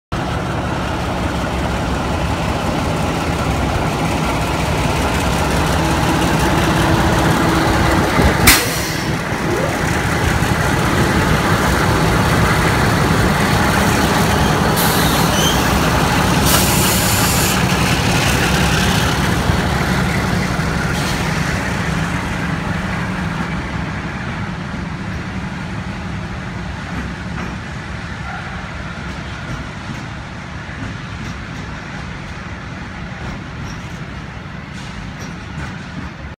MLWs in double consist at Afidnes Railway Station
BR234 attempting to uncouple unsuccessfully coal loaded wagon